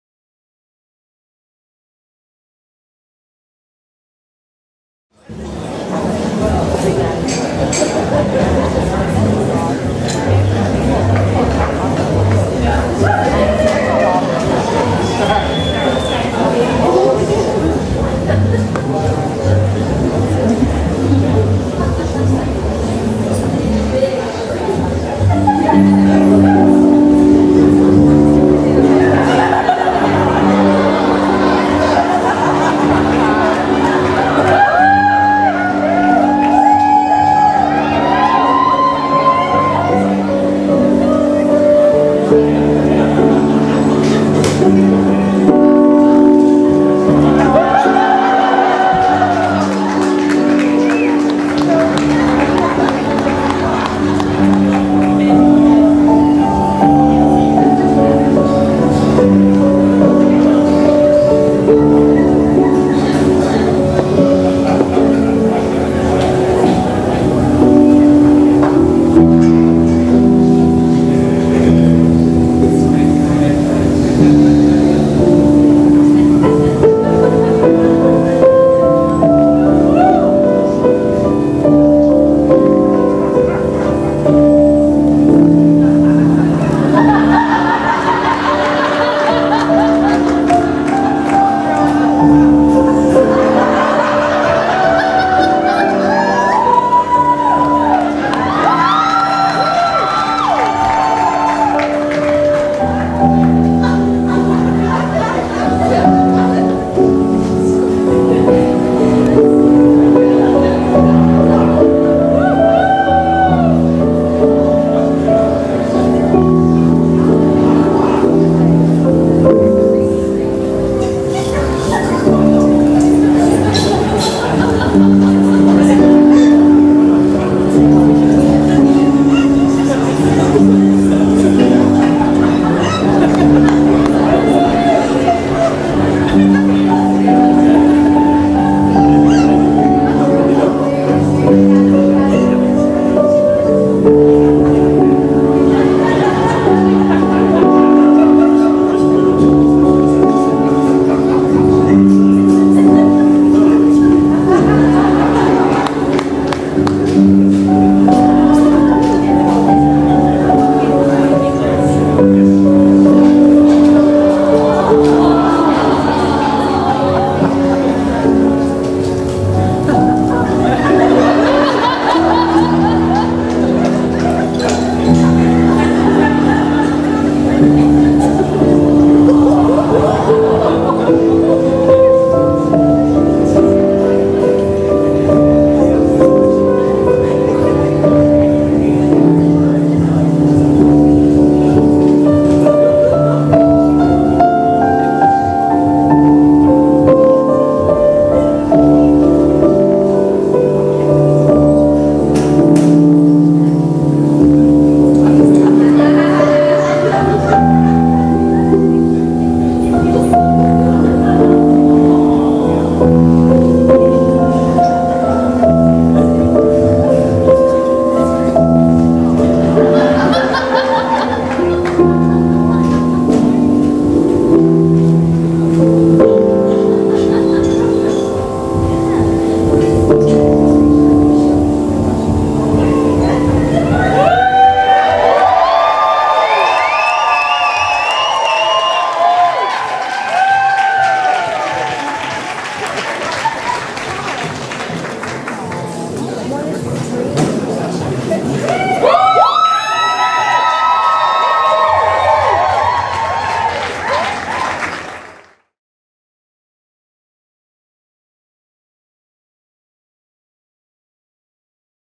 Toronto Burlesque Festival 2015 - The Lost Toys - Coco Framboise
Welcome to the lost and found department! Set in a busy train station, this narrative cabar-play follows a day in the life of our station clerk as he discovers cheeky lost toys and attempts to repair and reunite them with their rightful owners

Performer:
Coco Framboise